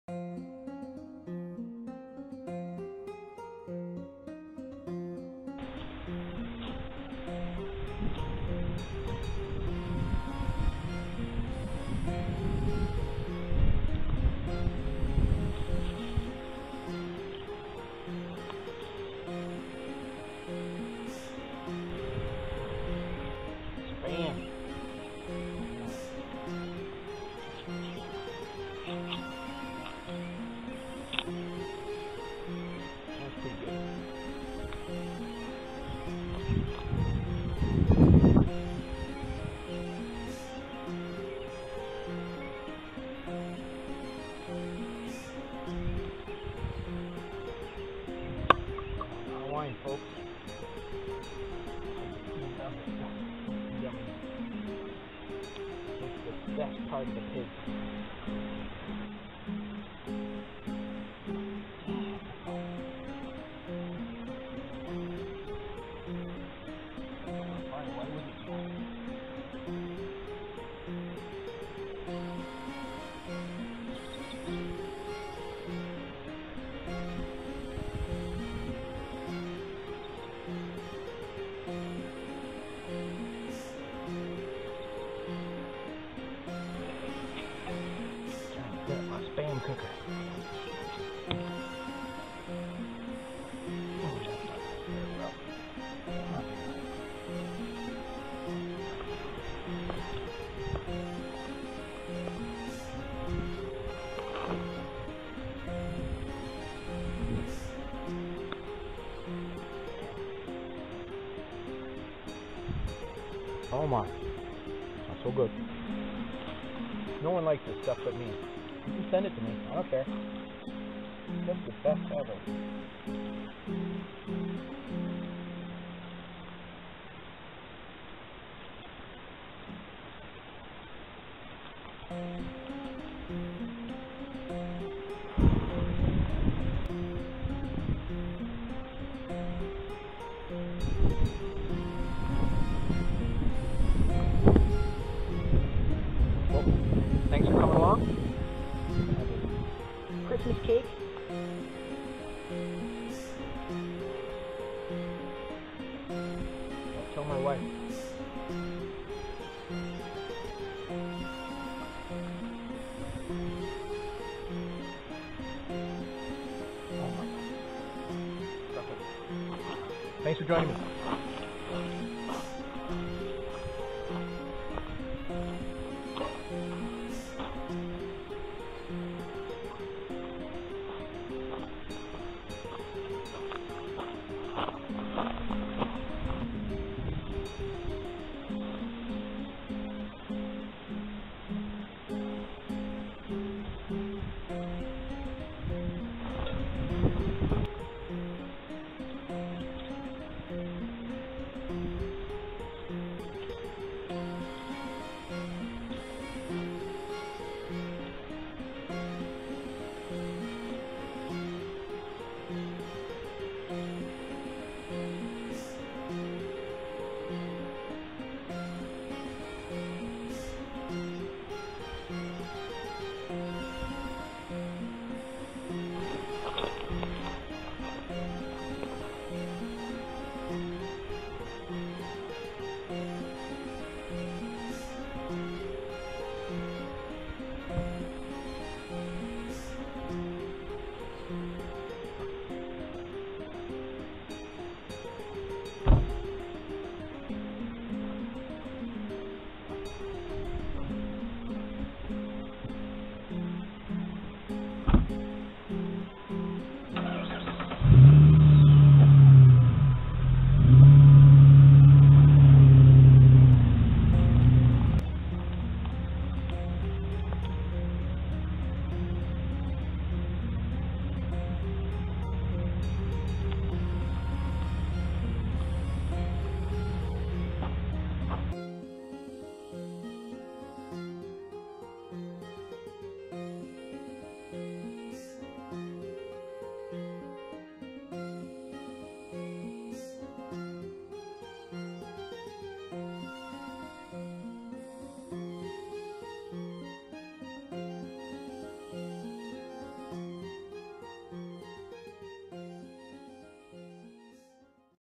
Tea and Lunch in old gravel pit
I found an old gravel pit that people have been sighting in rifles as I found lots of brass all around. also found a lot of moose tracks so I decided to make some tea and some lunch and enjoy the sunny day